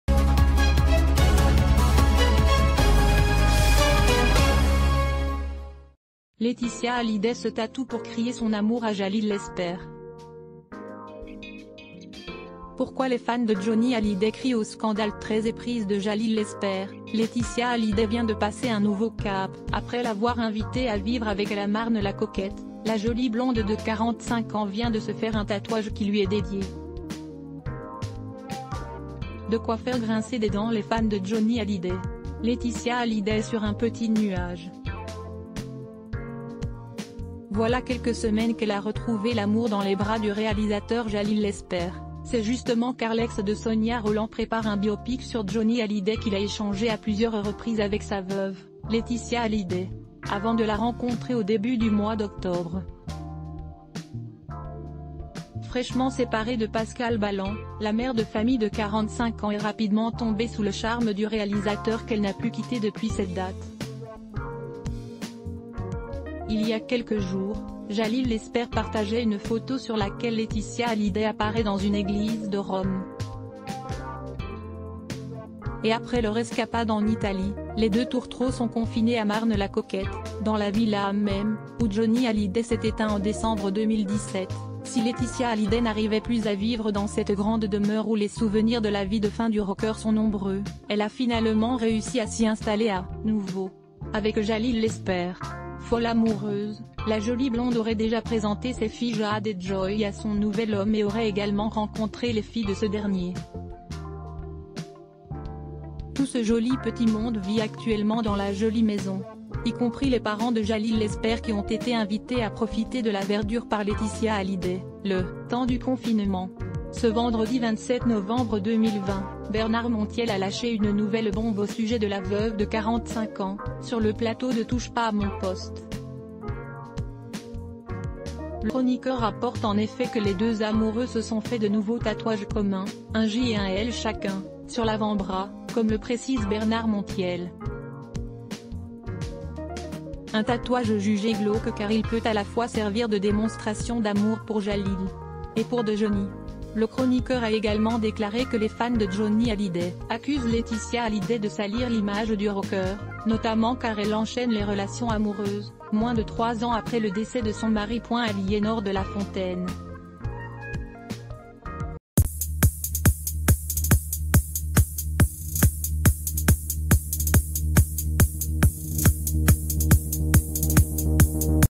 Laeticia Hallyday se tatoue pour crier son amour à Jalil Lespert... pourquoi les fans de Johnny...
Laeticia Hallyday se tatoue pour crier son amour à Jalil Lespert... pourquoi les fans de Johnny Hallyday crient au scandale

Laeticia Hallyday se tatoue po...